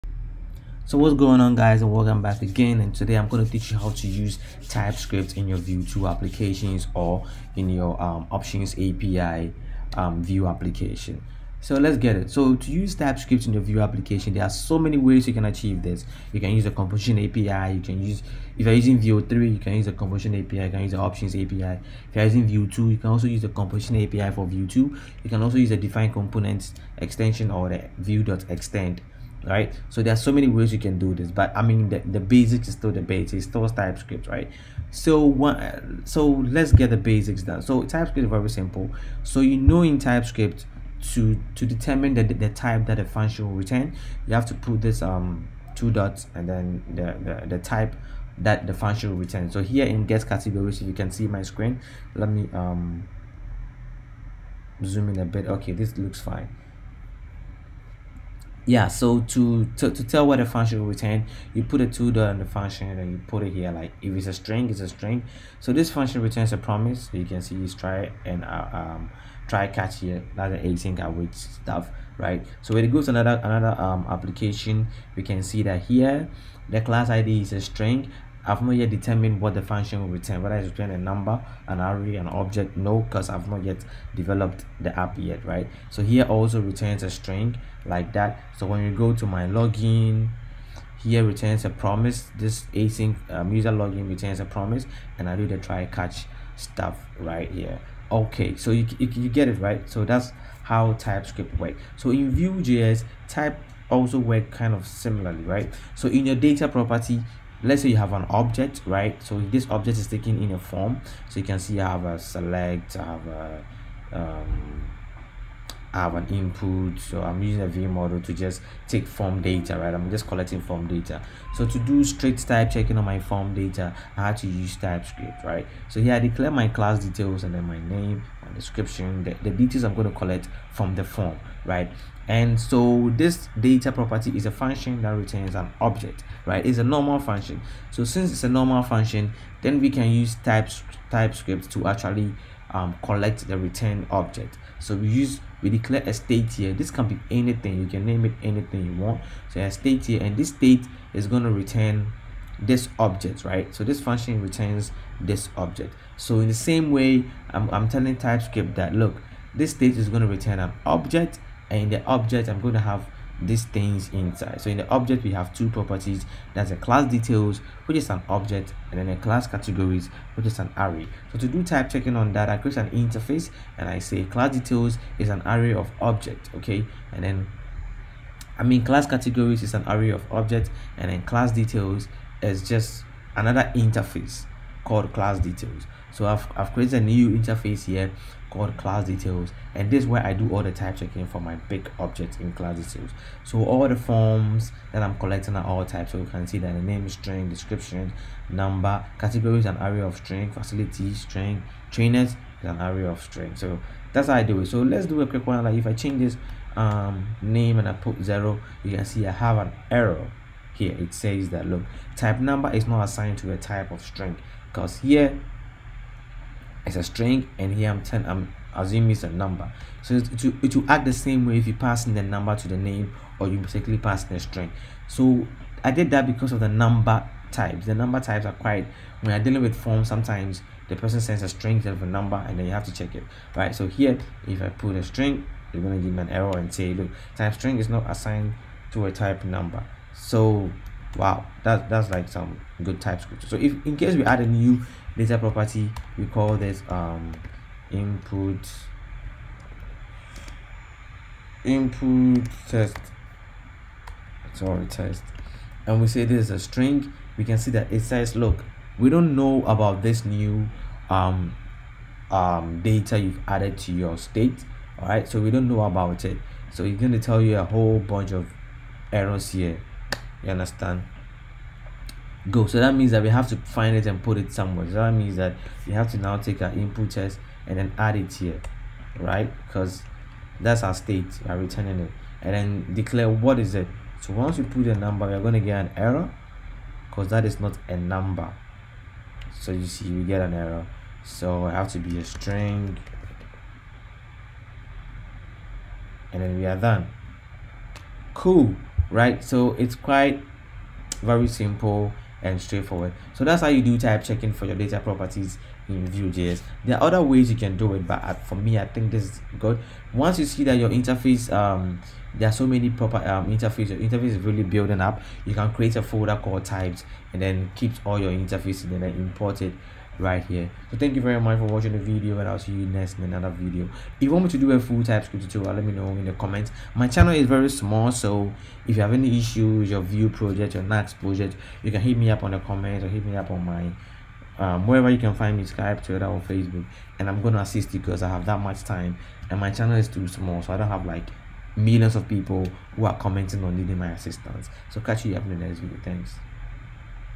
Vue Typescript - How to type check your Vue data properties with Typescript
In this video, I show you how to use typescript and Vue to type-check your vue data properties and state using Nuxt js